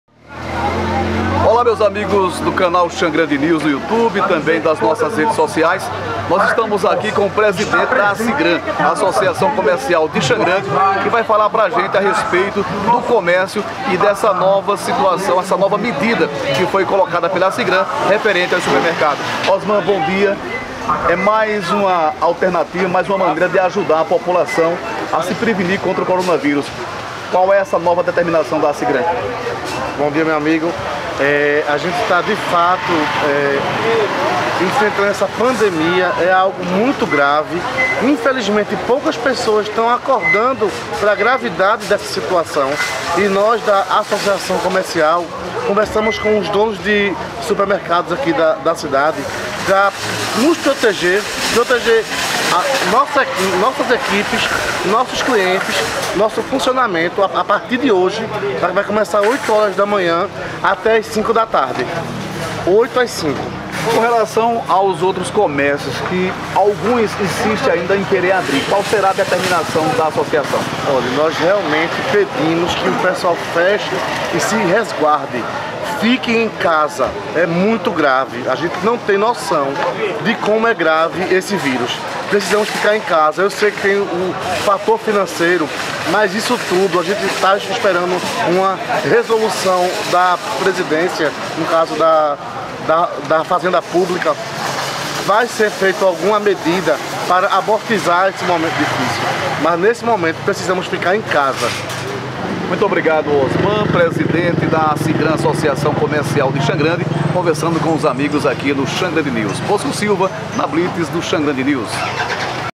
Acigran anuncia novo horário de funcionamento de supermercados
Agora, estabelecimentos comerciais irão abrir das 8h às 17h, como forma de evitar a aglomeração de pessoas.